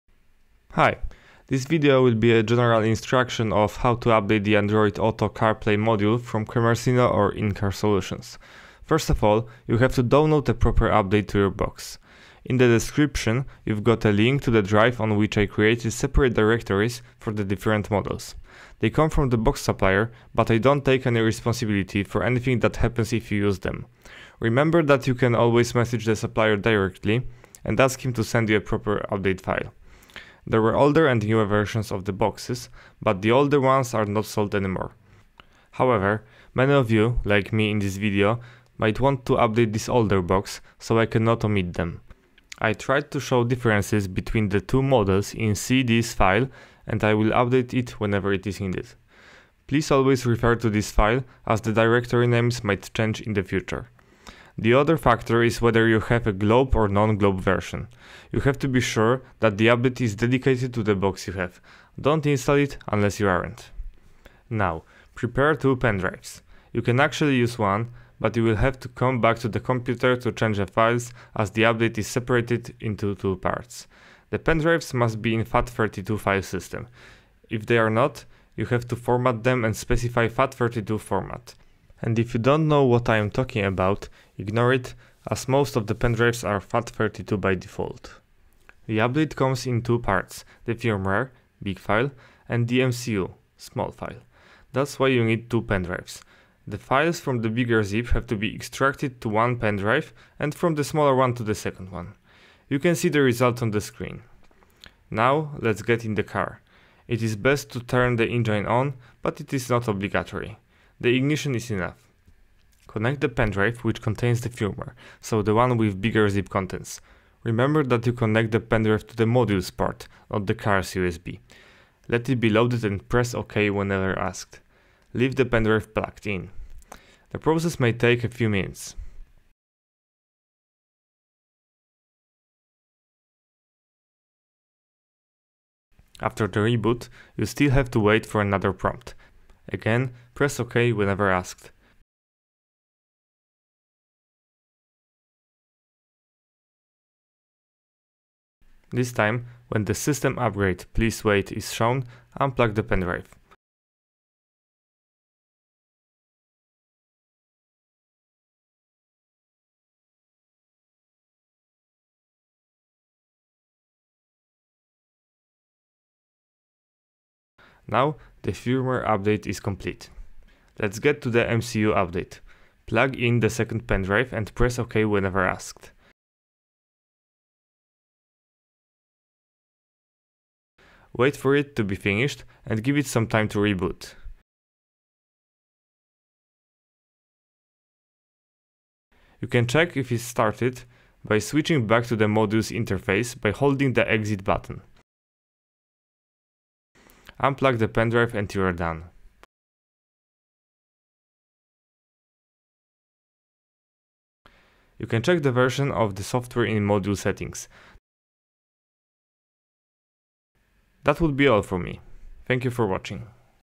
How to Update Volvo Android Auto/CarPlay module? (V70 III/XC70 II/S80 II/V60/XC60/S60 II/V40 II)
This video cointains instructions on how to update Android Auto/CarPlay Box in P3 Volvos (V70 III/XC70 II/S80 II/V60/XC60/S60 II/V40 II).

If you are unsure, please reach out to the seller.

❓ If you have any questions ask in comments or write a message directly to the seller.